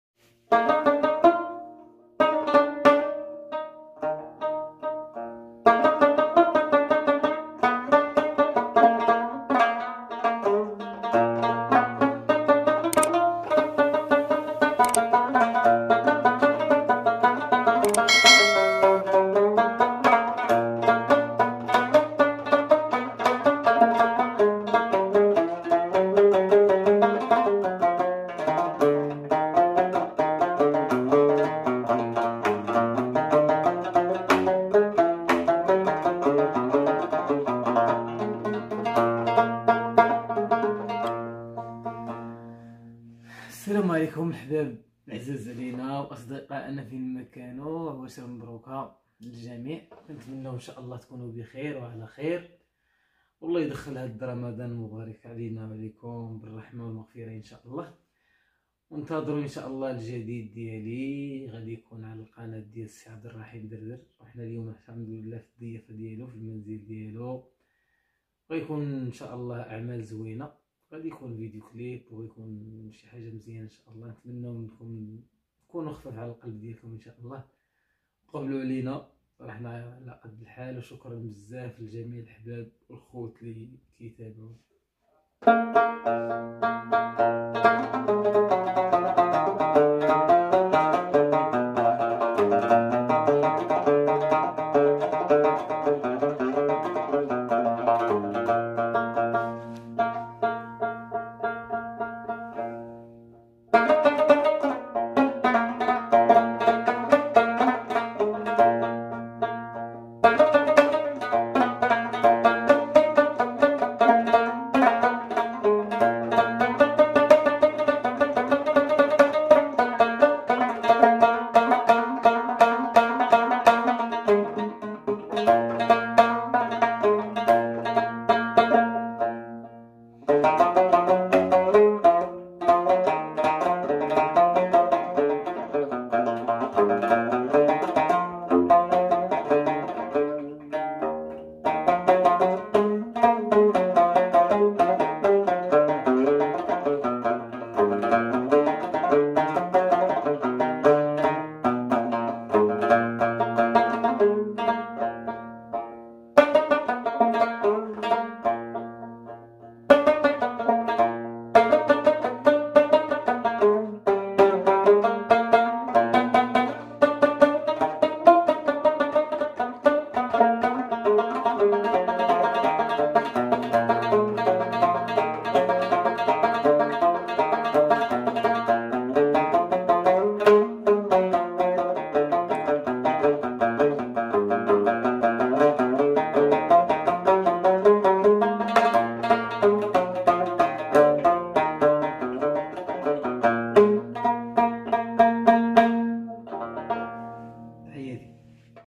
جلسة فنية عند خويا عبد الرحيم دردر قبلو علينا وترقبو الجديد الفنان أحمد الناصري
جلسة فنية مع الفنان أحمد الناصري في سبت جزولة عند خويا عبد الرحيم دردر  ❤️ وترقبو الجديد أن شاء الله ❤️

جلسة_فنية_مع الفنان_أحمد_الناصري_وترقبو_الجديد#